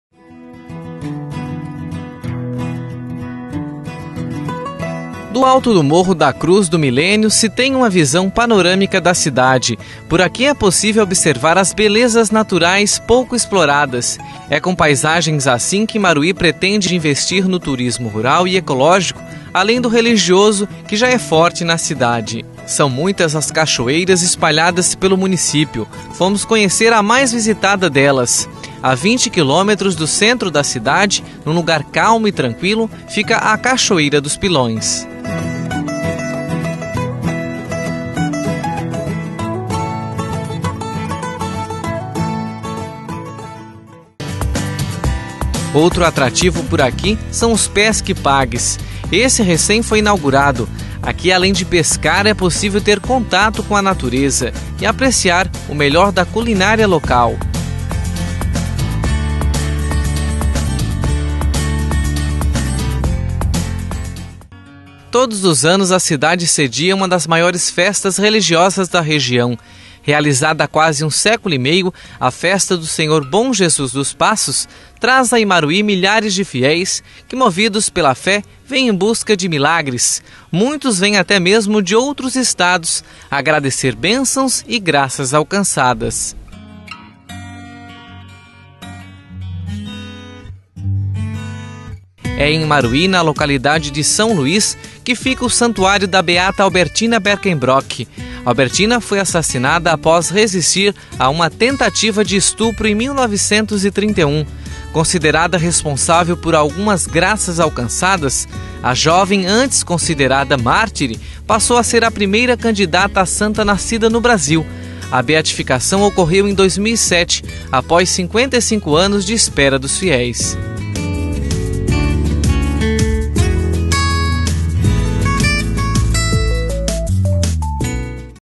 Imaruí: turismo religioso atrai turistas de toda região
A cidade de Imaruí conta com belas cachoeiras e ricas paisagens. Além disso, o turismo religioso, move os fiéis da cidade e da região, atraindo milhares de pessoas todos os anos